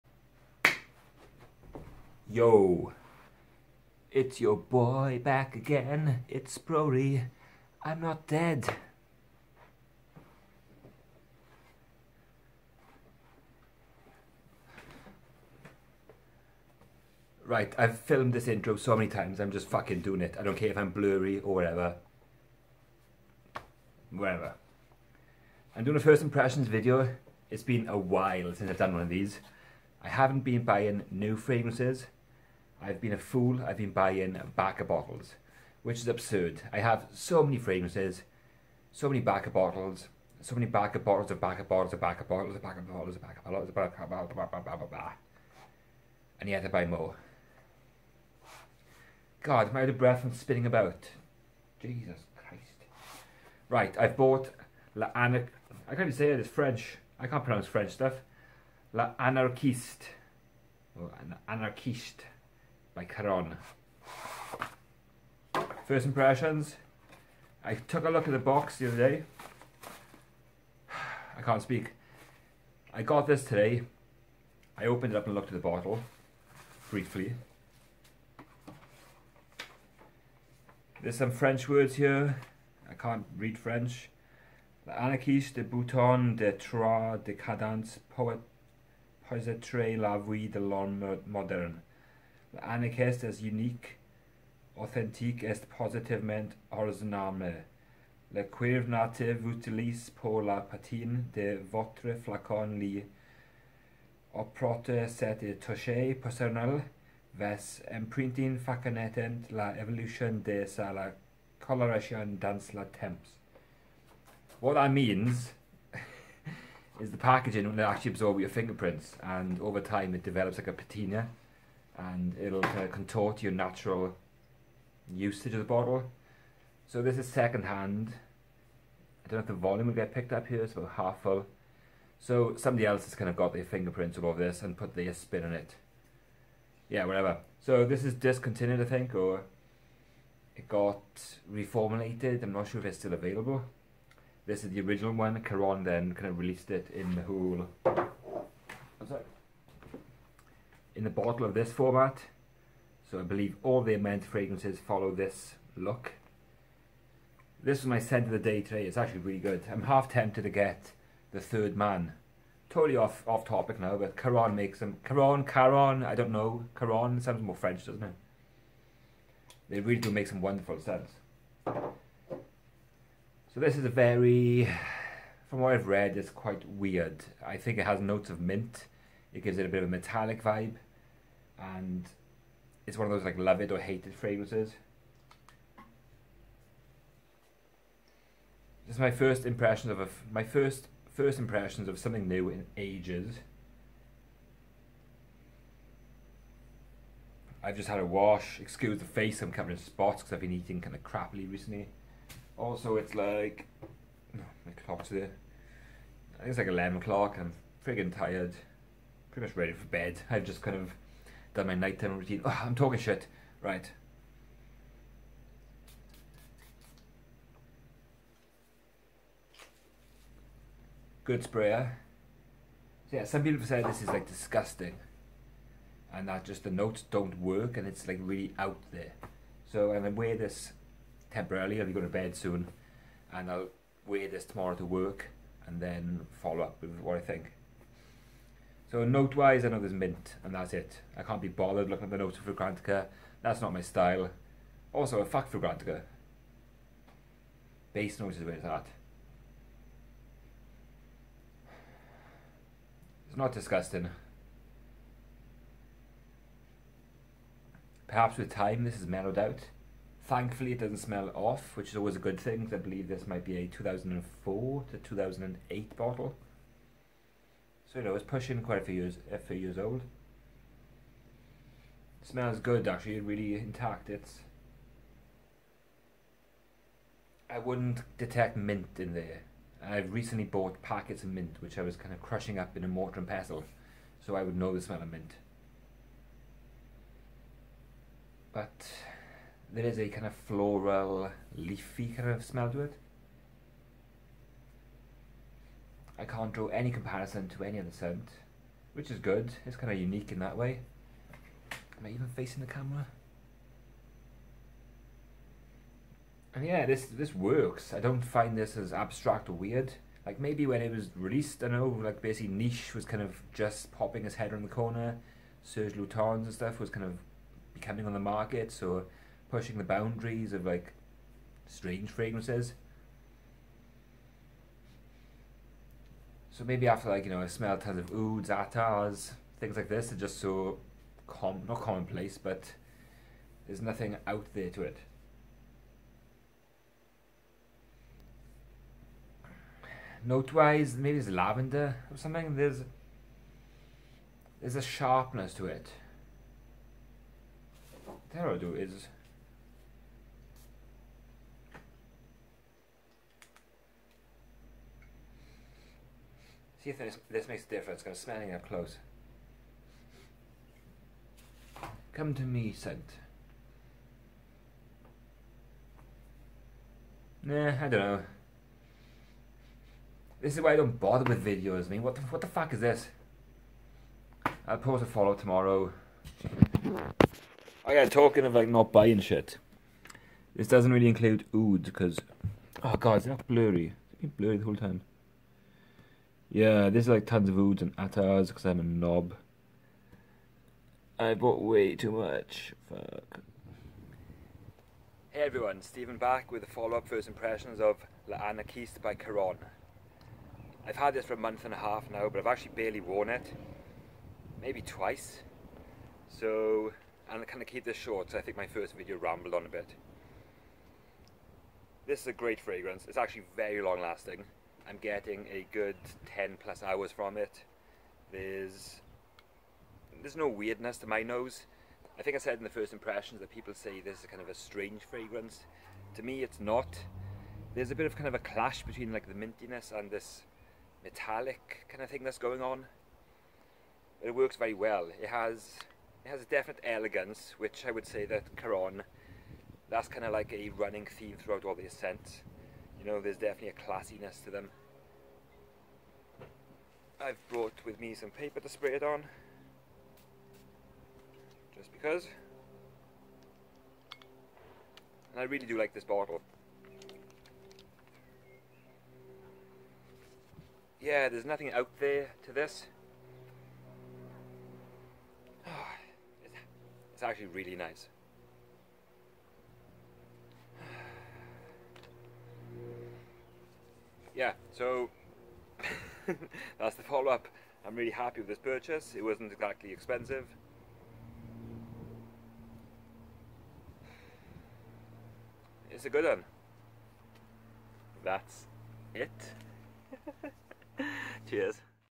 L'Anarchiste by Caron | First Impressions
Filmed over a month and half apart - here's my crappy take on this great fragrance. I filmed this before my last first impressions, so when I say it's been a while - that was true at the time of filming!